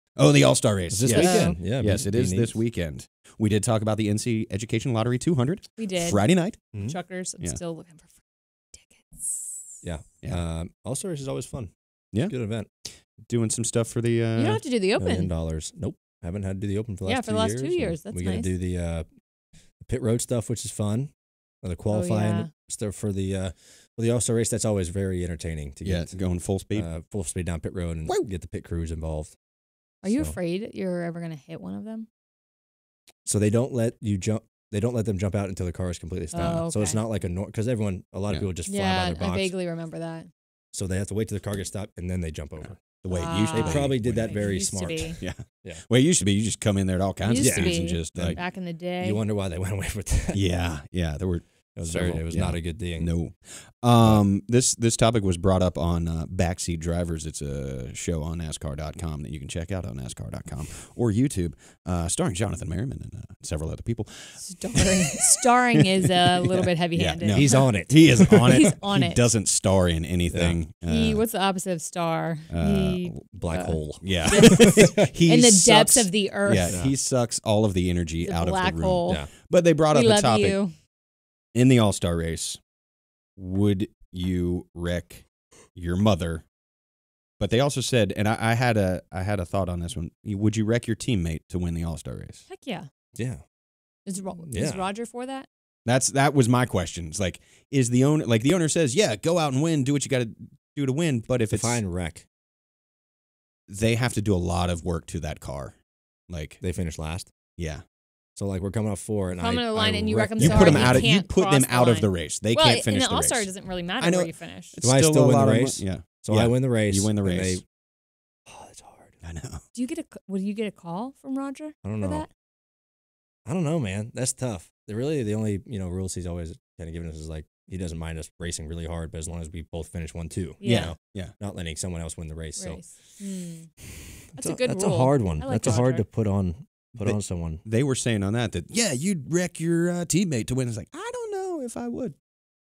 Podcast: Blaney talks wrecking teammates for $1 million
Ryan Blaney and the GCOE crew talk about wrecking teammates to win the Monster Energy All-Star Race.

Play every weekend!